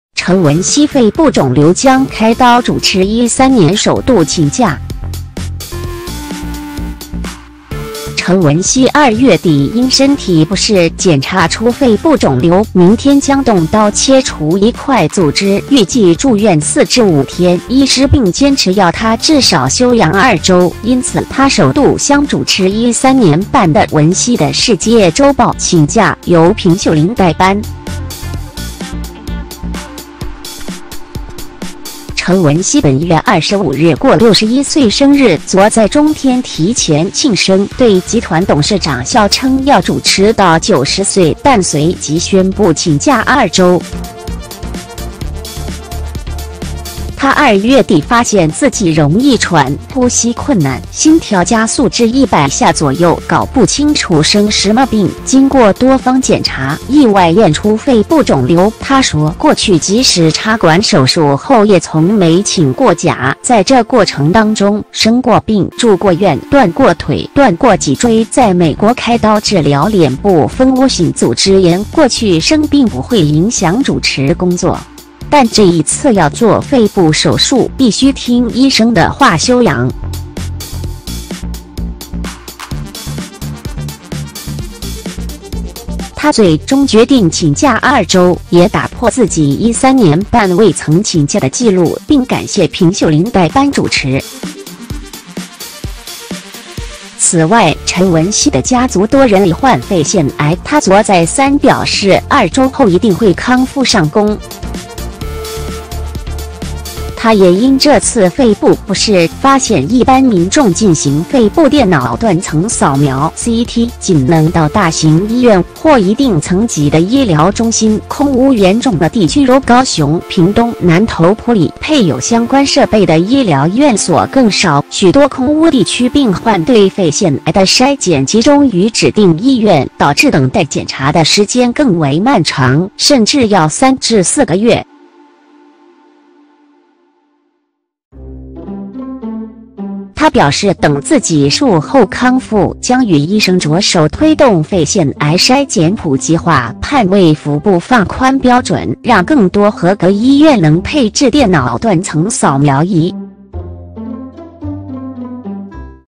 陳文茜肺部腫瘤將開刀 主持13年首度請假
陳文茜2月底因身體不適，檢查出肺部腫瘤，明天將動刀切除一塊組織，預計住院4至5天，醫師並堅持要她至少休養2周，因此她首度向主持13年半的《文茜的世界周報》請假，由平秀琳代班。
陳文茜本月25日過61歲生日，昨在中天提前慶生，對集團董事長笑稱要主持到90歲，但隨即宣布請假2周。她2月底發現自己容易喘，呼吸困難，心跳加速至100下左右，搞不清楚生什麼病，經過多方檢查，意外驗出肺部腫瘤，她說：「過去即使插管手術後也從沒請過假，在這過程當中，生過病、住過院、斷過腿、斷過脊椎、在美國開刀治療臉部蜂窩性組織炎，過去生病不會影響主持工作，但這一次要做肺部手術，必須聽醫生的話休養。」她最終決定請假2周，也打破自己13年半未曾請假的紀錄，並感謝平秀琳代班主持。此外，陳文茜的家族多人罹患肺腺癌，她昨再三表示，2周後一定會康復上工。
她也因這次肺部不適，發現一般民眾進行肺部電腦斷層掃描（CT），僅能到大型醫院，或一定層級的醫療中心，空汙嚴重的地區如高雄、屏東、南投埔里，配有相關設備的醫療院所更少，許多空汙地區病患對肺腺癌的篩檢集中於指定醫院，導致等待檢查的